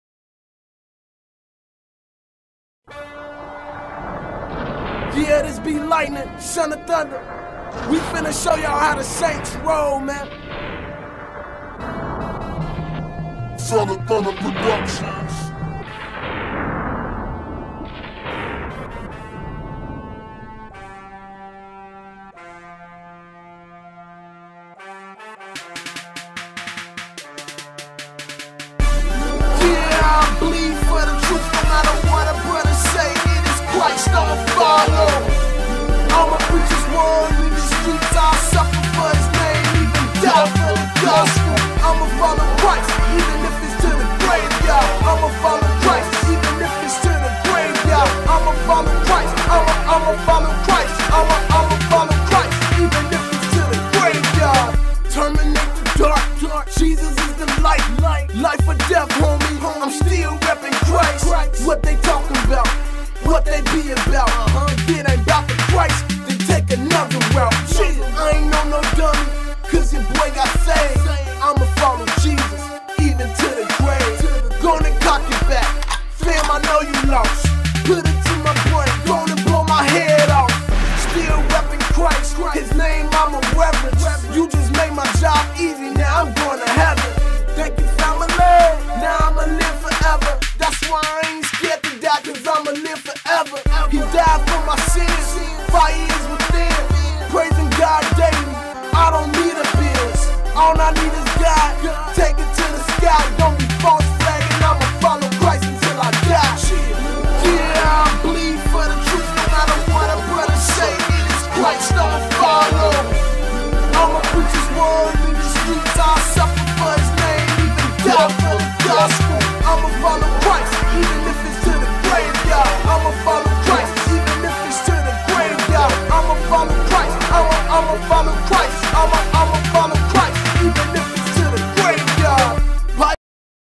"Lightning the son of Thunder" on the DrCal Hour of Insight"
Lightning son of Thunder, is not just another rap artist. There is a mission in his heart, when you hear the words and the powerful delivery, then you will realize that something
has changed him and that great love is what he wants to share. The Good News is given on many forms. This is our first Rap artist ever on Insight why? Because I was moved and inspired I want you to hear this! The Interview starts at 4PMEST with your host DrCal